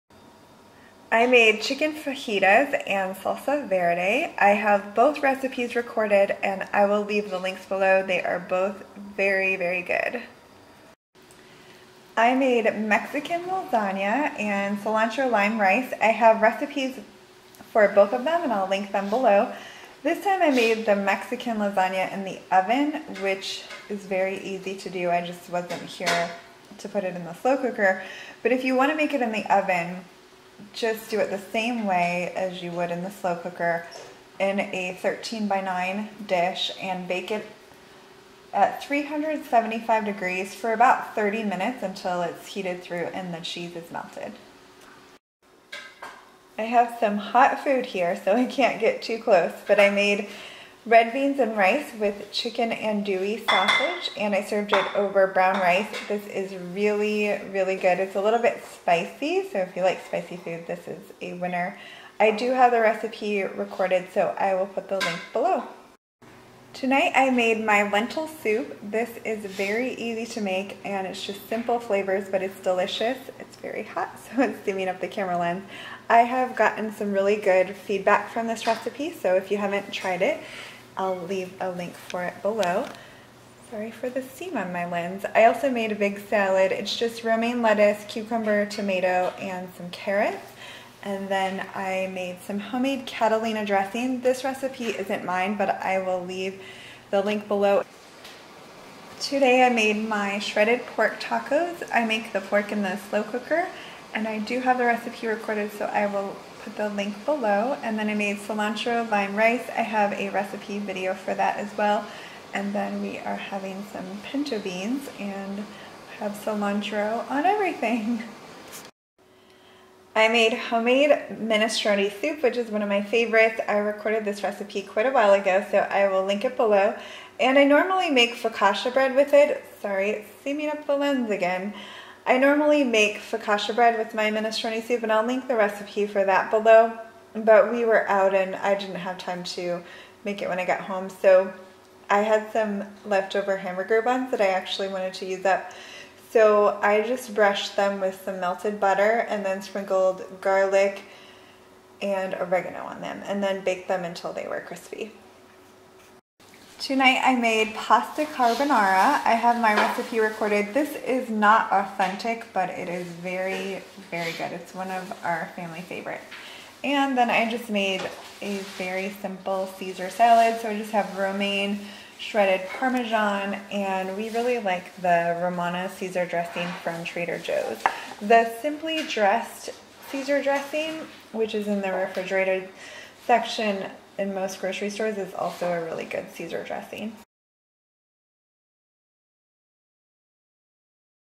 7 Delicious Dinners with Recipes
I picked seven delicious dinners that I've made over the past couple weeks and I have recipes for each of them linked below. Meal planning made easy!

💙Amazon Store

Save 25% by using code JEN25 (menu plan, recipes and grocery lists)